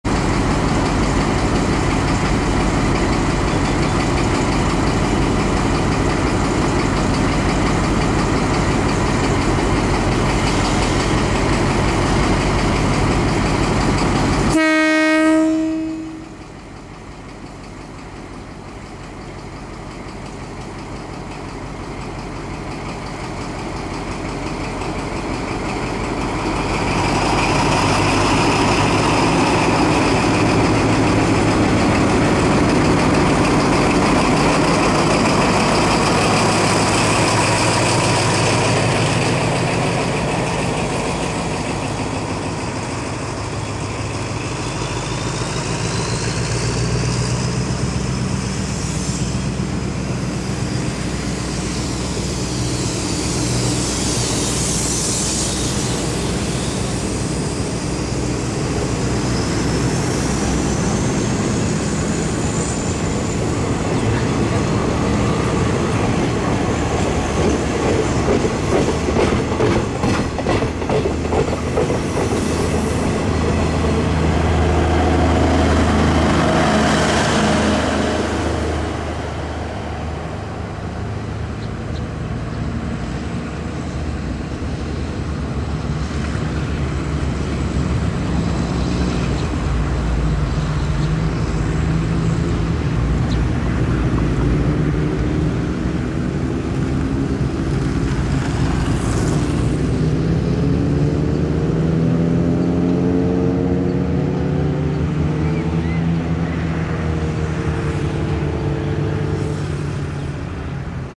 2011.01.11 1047h Kuala Kangsar, Perak
Southbound 1Dn Ekspres Rakyat from Butterworth continuiing with its journey to Kuala Lumpur after a brief stop at this royal town.